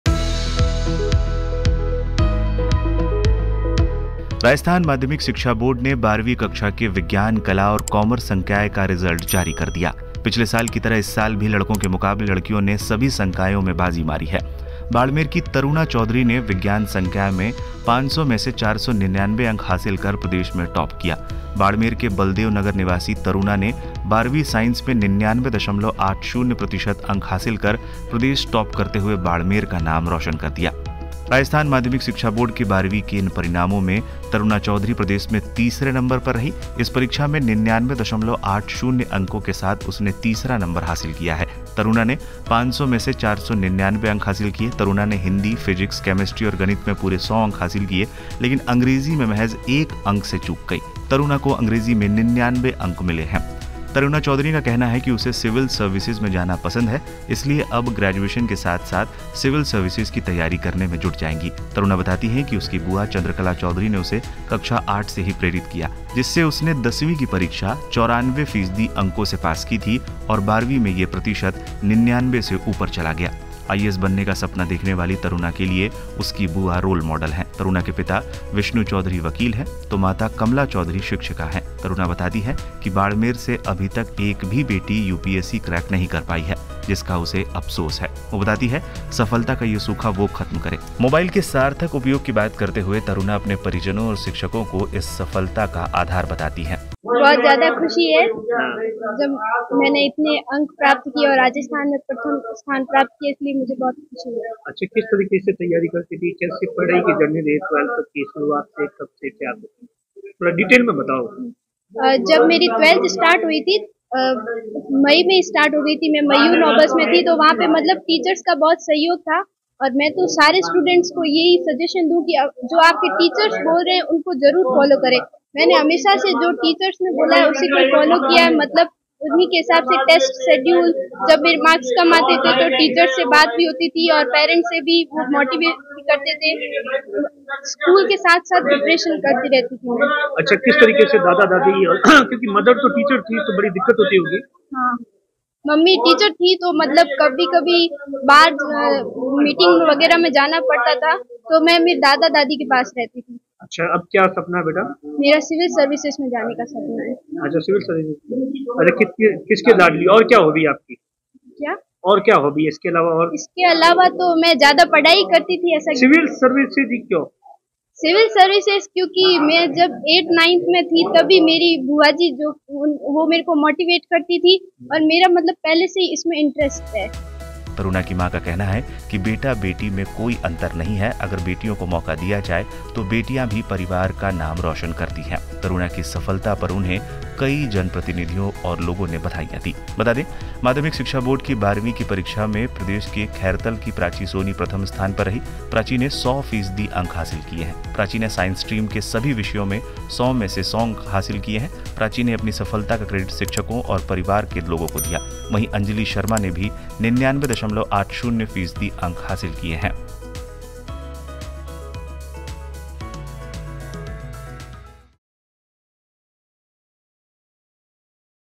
RBSE Board Result 2024: Taruna Chaudhary का 01 नबंर कहां कटा, देखिए मार्कशीट | Rajasthan Result
राजस्थान माध्यमिक शिक्षा बोर्ड ने 12वीं कक्षा के विज्ञान, कला और कॉमर्स संकाय का रिजल्ट जारी कर दिया है। पिछले साल की तरह इस साल भी लड़कों के मुकाबले लड़कियों ने सभी संकायों में बाजी मारी है। बाड़मेर की तरुणा चौधरी ने विज्ञान संकाय में 500 में से 499 अंक हासिल कर प्रदेश के टॉप किया है।